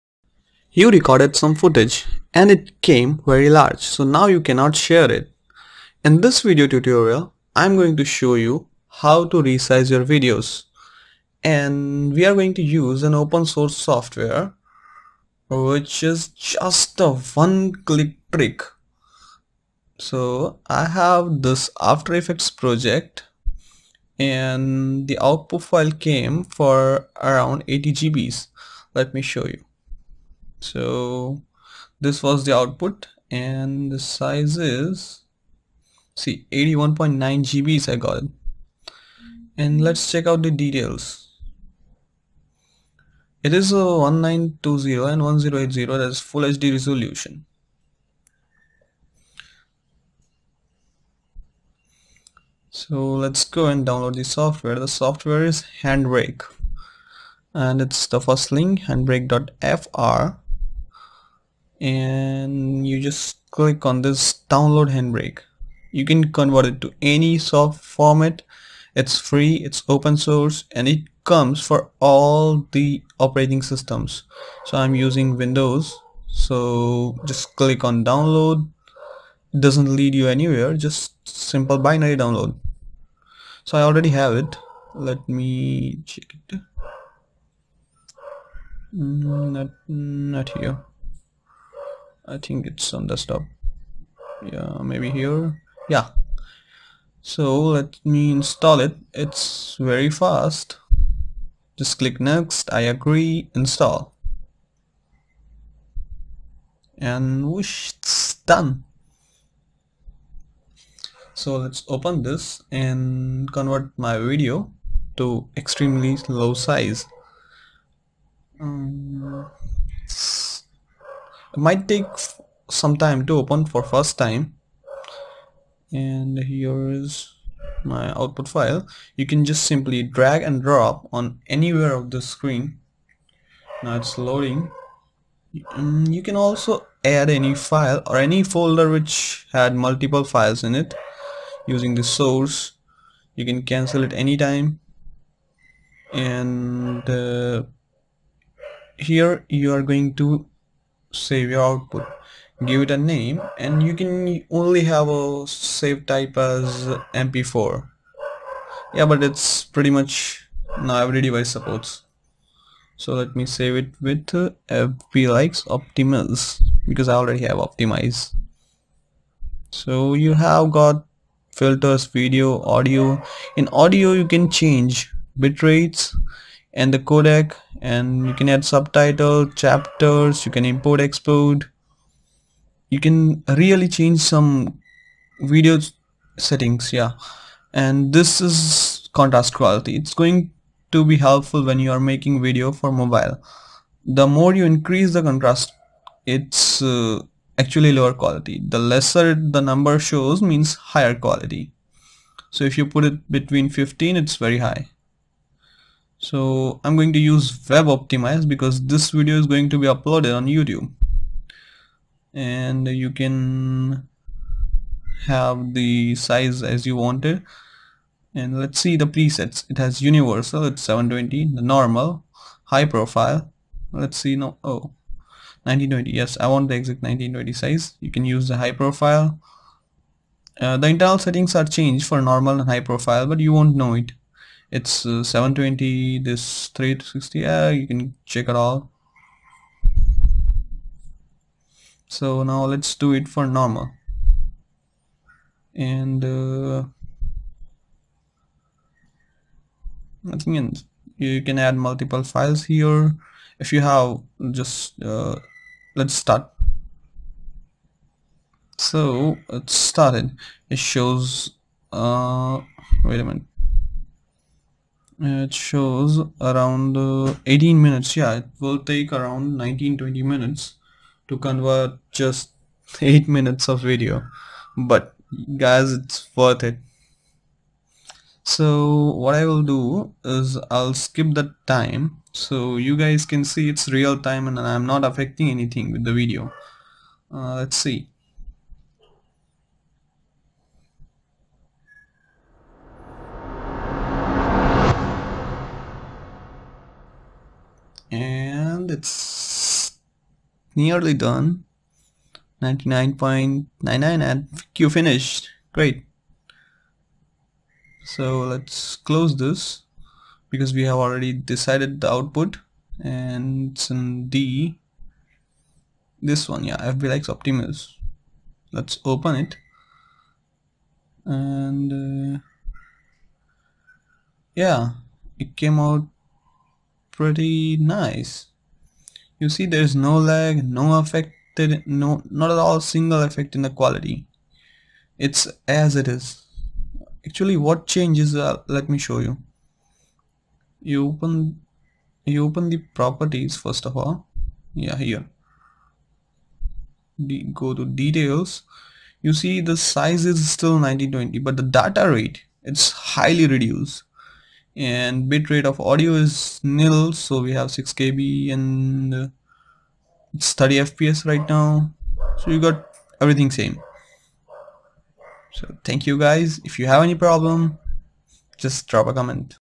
Best Way to Reduce Video File Size without loosing Quality or Resolution
In this video tutorial, I've discussed the easiest way to reduce the size of a video (Not to be confused with its resolution) . Using an open-source (free) software called 'HandBreak'. which is the best software for video size reduction, check out its google reviews too.

You can use to software to optimise the size of the video in any format and it's available for nearly all OS. it provides many other useful functions as a video editor - to change the resolution, add or remove audio, modify or add subtitles, change the quality of the video.

So, Enjoy the tutorial and learn how you can easily turn your recorded large size video files into a shareable video with optimised size that too without affecting its quality.
Copyrights : Soumy Nayak | Forever Suspicious Videos